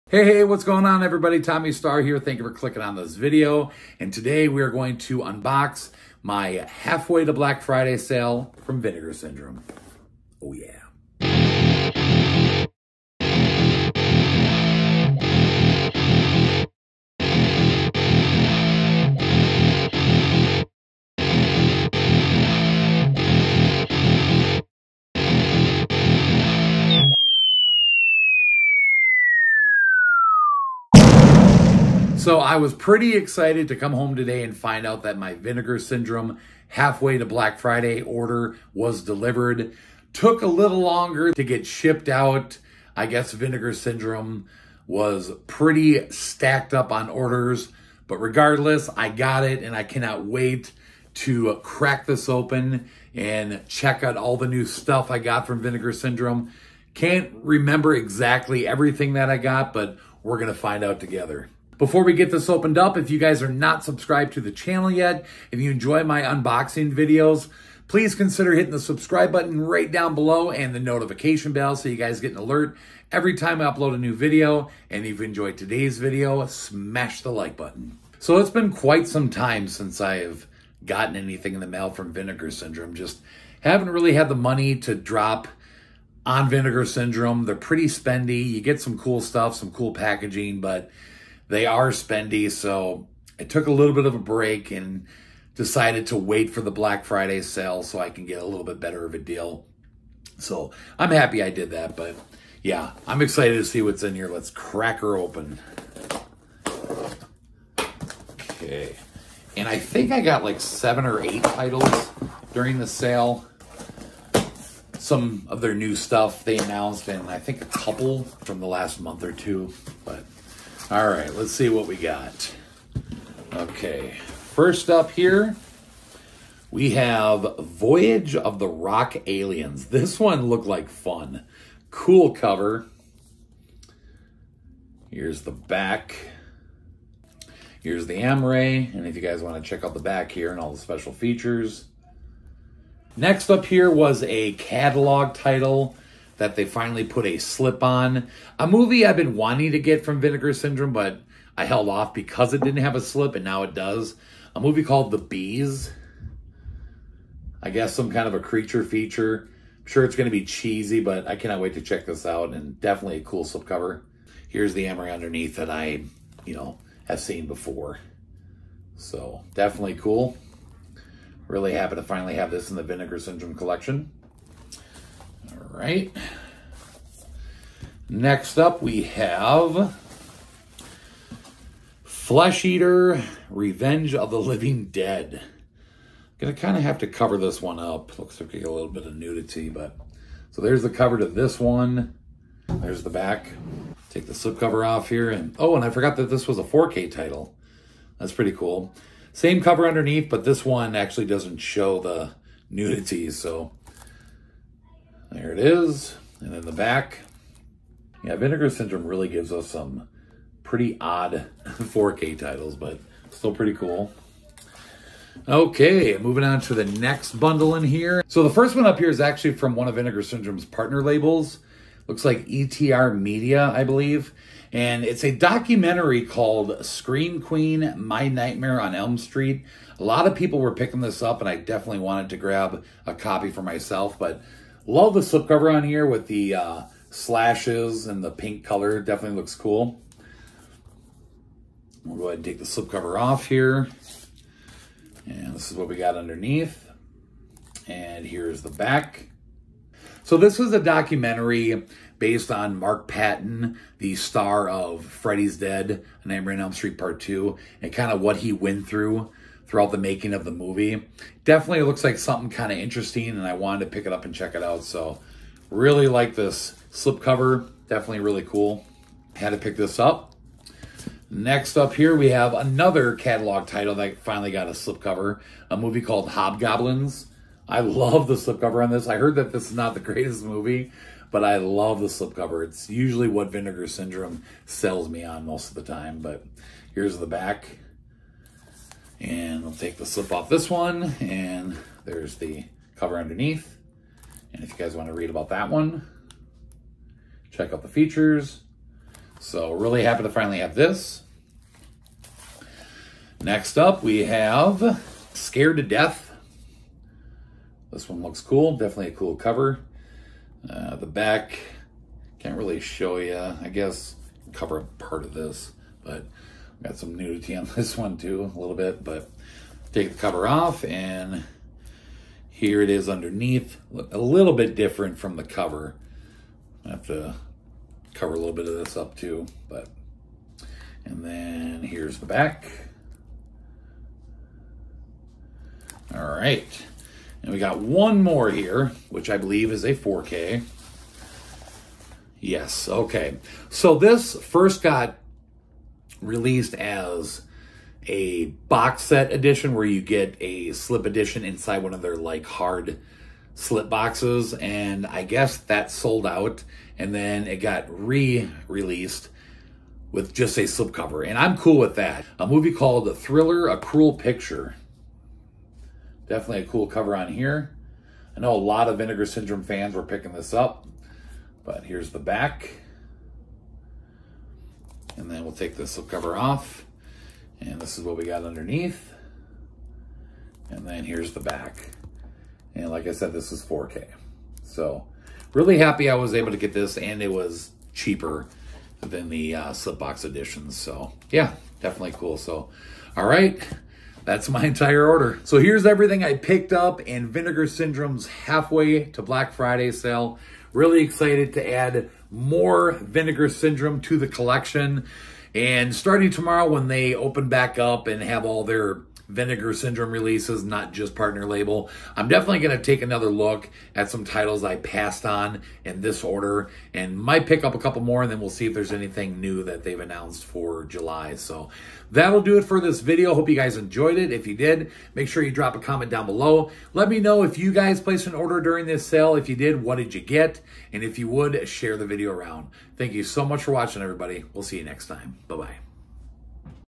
Unboxing Ep. #219 (Vinegar Syndrome Halfway to Black Friday Sale 2022!!!)
I finally got my Vinegar Syndrome “Halfway to Black Friday Sale 2022” order!!! Some really cool stuff!! Enjoy!

Angelsheavenlynailz

Keep It Keto